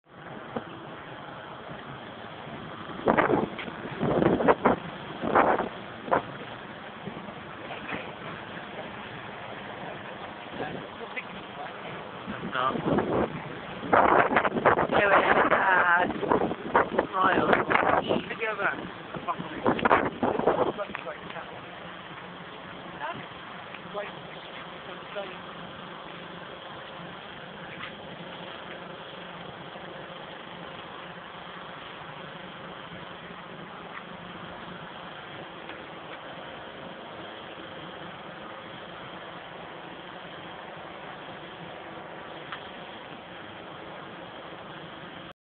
catamaran in whitstable harbour
My dad was really impressed by the size and quality of this catamran. It's small but look...